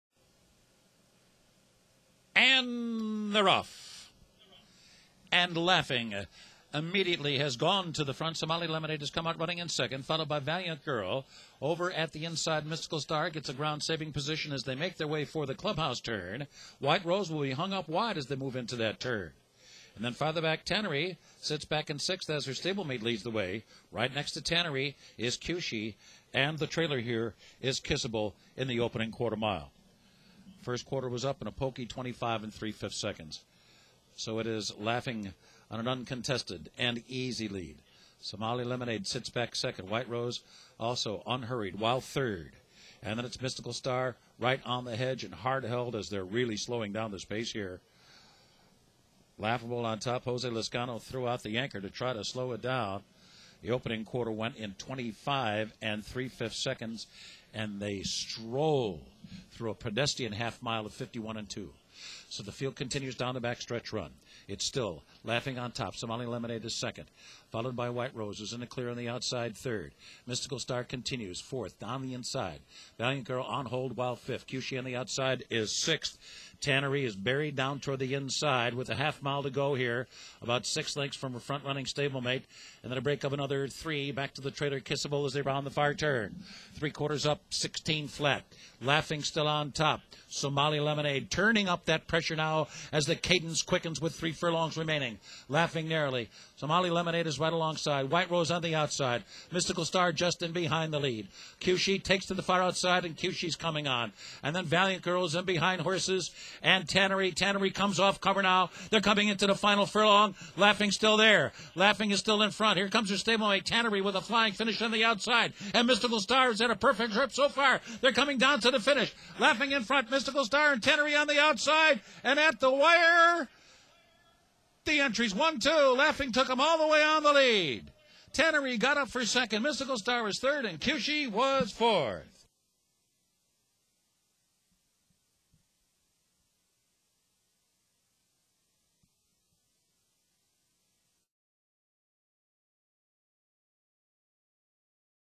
Laughing - 2013 Flower Bowl (G1)
Taking full advantage of being the lone speed horse in a field of eight, Laughing doled out creeping fractions through the early stages of the Grade 1, $600,000 Flower Bowl Invitational on Saturday and then turned on her speed late to win gate-to-wire by a half-length at Belmont Park.

The victory in the 1 ¼-mile fixture on the inner turf over her entrymate Tannery came in a time of 2:02.34 and placed Laughing squarely in the equation for the Eclipse Award for Champion Turf Female.

Laughing ran through an opening half-mile in 51.46 seconds and six furlongs in 1:16.02. She came home the final half-mile in 46.32 seconds.